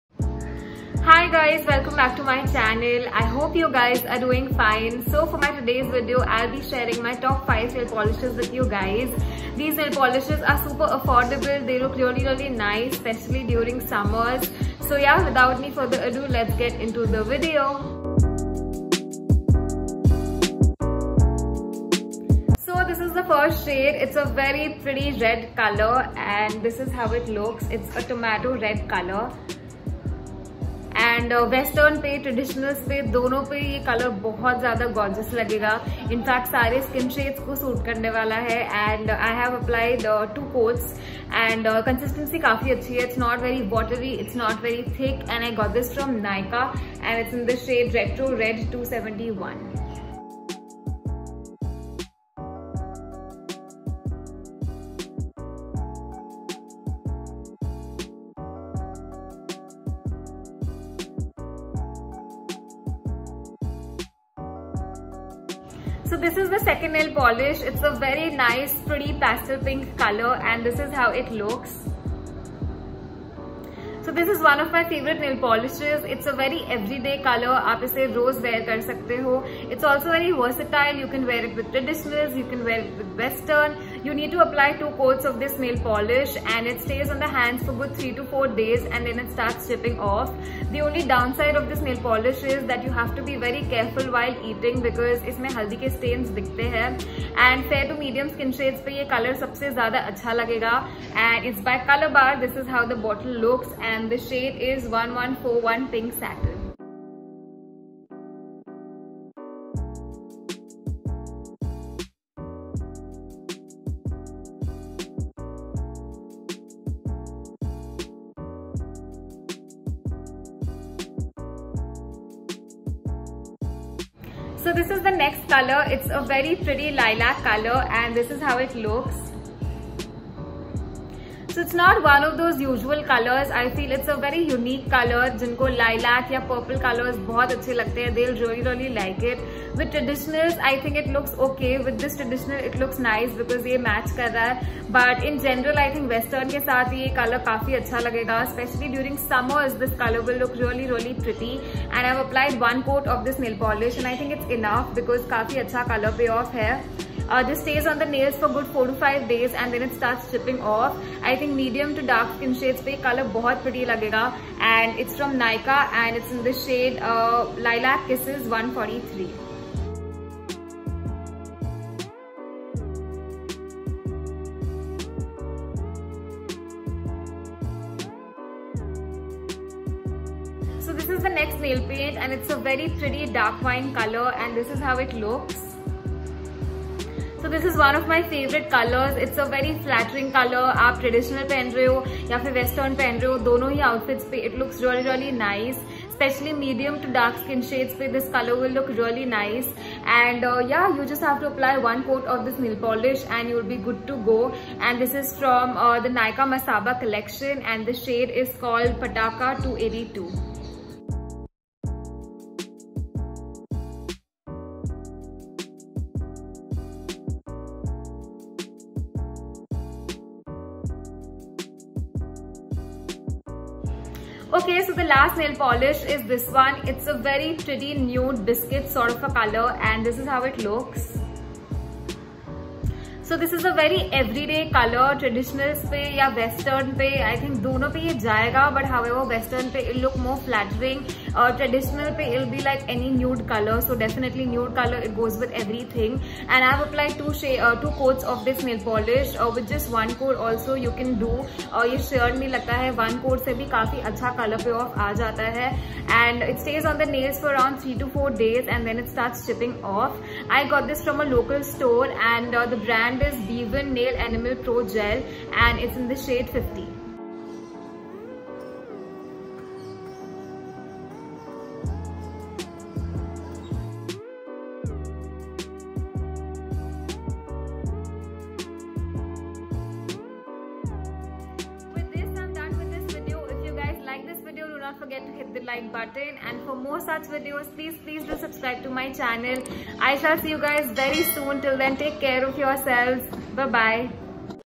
My Nail Polish Collection | Nykaa Nail Polish
My Nail Polish Collection | Nykaa Nail Polishes

Nail Polishes included in this video are :

1. Masaba by Nykaa Nail Enamel - Pataka 282 On Nykaa

2. Nykaa Nail Enamel Polish - Lilac Kisses 143 On Nykaa

3. Colorbar Nail Lacquer - Pink Satin On Nykaa

4. Nykaa Wedding Edition Nail Enamel Polish - Retro Red 271 On Nykaa